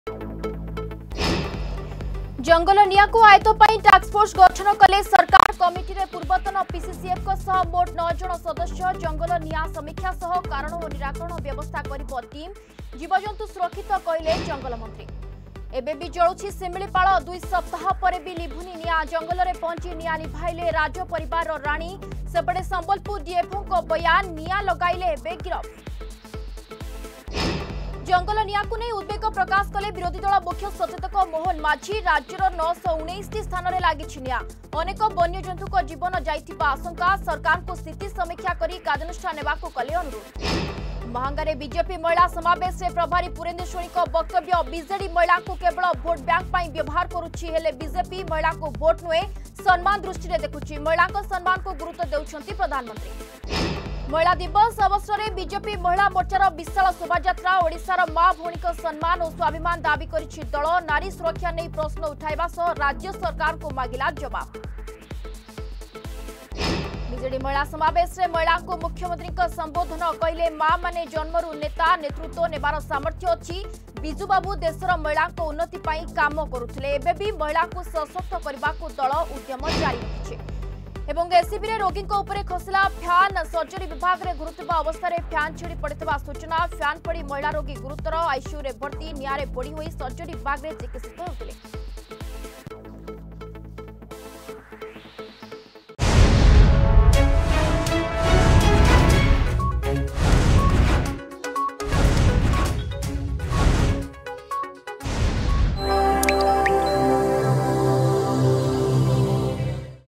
Headlines 4pm
ଜଙ୍ଗଲ ନିଆଁକୁ ଆୟତ୍ତ ପାଇଁ ଟାସ୍କ ଫୋର୍ସ ଗଠନ କଲେ ସରକାର...

ଏବେବି ଜଳୁଛି ଶିମିଳିପାଳ... 2 ସପ୍ତାହ ପରେ ବି ଲିଭୁନି ନିଆଁ...

ଜଙ୍ଗଲ ନିଆଁକୁ ନେଇ ଉଦବେଗ ପ୍ରକାଶ କଲେ ବିରୋଧୀଦଳ ମୁଖ୍ୟ ସଚେତକ ମୋହନ ମାଝୀ.

ମାହାଙ୍ଗାରେ ବିଜେପି ମହିଳା ସମାବେଶରେ ପ୍ରଭାରୀ ପୁରନ୍ଦେଶ୍ୱରୀଙ୍କ ବକ୍ତବ୍ୟ...

ମହିଳା ଦିବସ ଅବସରରେ ବିଜେପି ମହିଳା ମୋର୍ଚ୍ଚାର ବିଶାଳ ଶୋଭାଯାତ୍ରା.

ବିଜେଡ଼ି ମହିଳା ସମାବେଶରେ ମହିଳାଙ୍କୁ ମୁଖ୍ୟମନ୍ତ୍ରୀ ସମ୍ବୋଧନ...

ଏବଂ ଏସସିବିରେ ରୋଗୀଙ୍କ ଉପରେ ଖସିଲା ଫ୍ୟାନ୍.